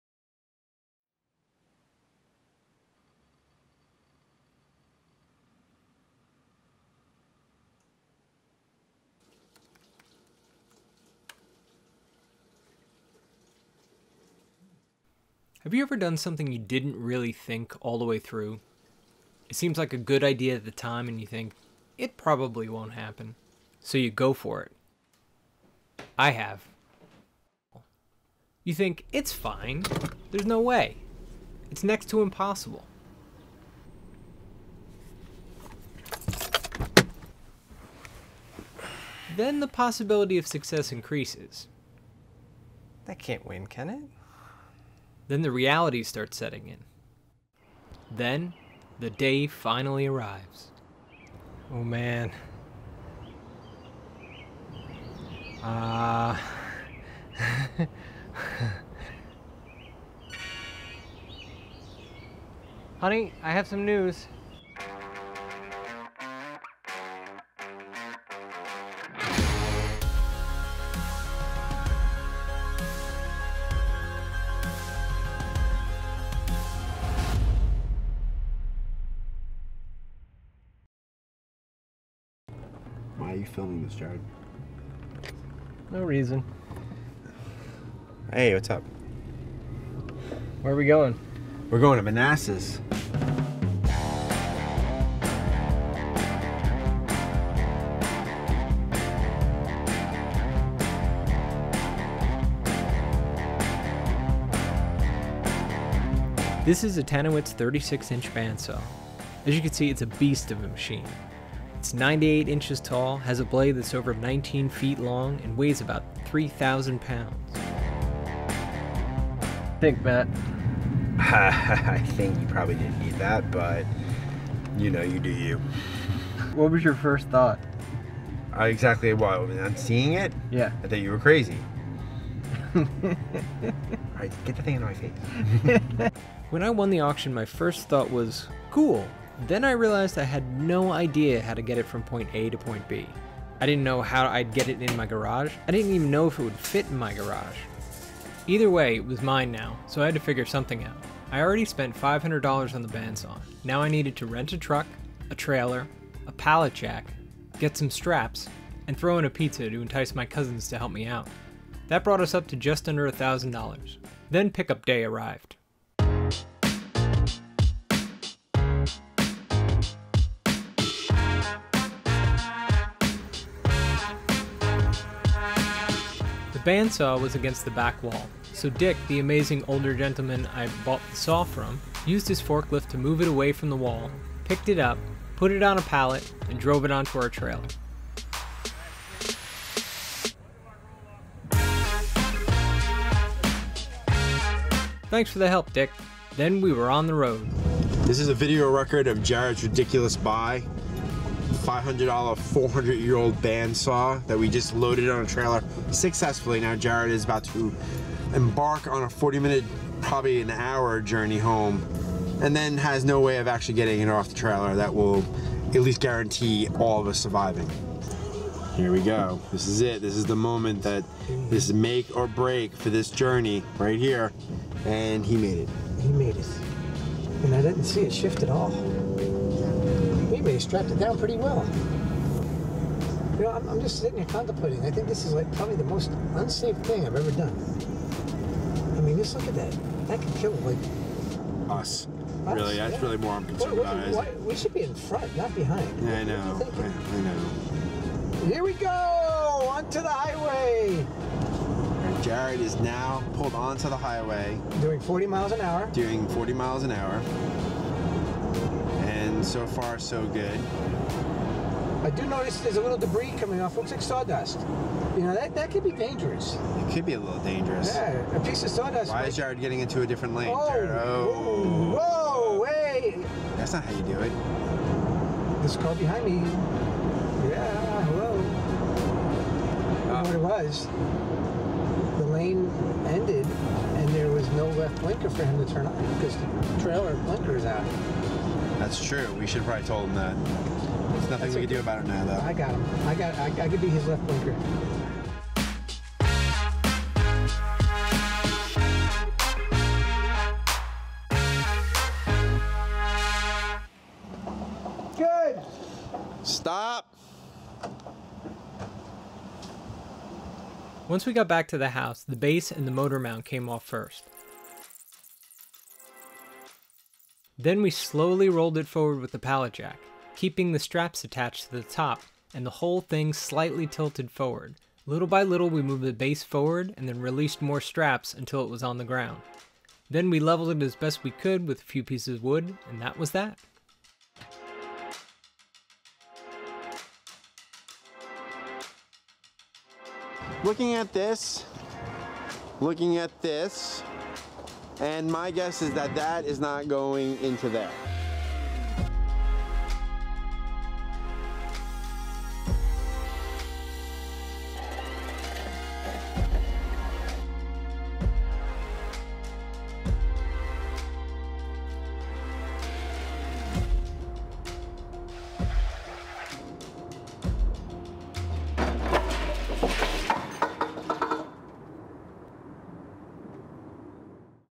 Acquiring the 1943 36" Tannewitz Bandsaw
I get the bandsaw I never thought I'd get. A 36" Tannewitz from 1943.